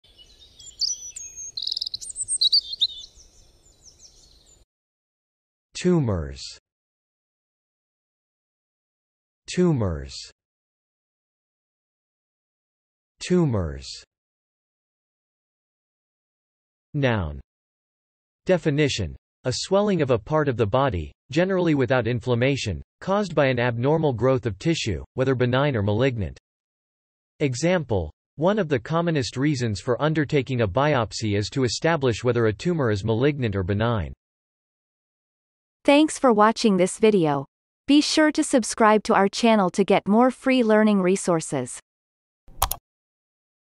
How to pronounce tumors - Vocab Today
The pronunciation of the word tumors in Amercian accent is demonstrated in this video. If the word is from another language, such as brand name, it will be pronounced in its original language. Based on the word frequency, this is of 3 stars difficulty.

The word tumors has the following meanings:
1. (noun)
Definition : A swelling of a part of the body, generally without inflammation, caused by an abnormal growth of tissue, whether benign or malignant.
Example : One of the commonest reasons for undertaking a biopsy is to establish whether a tumour is malignant or benign.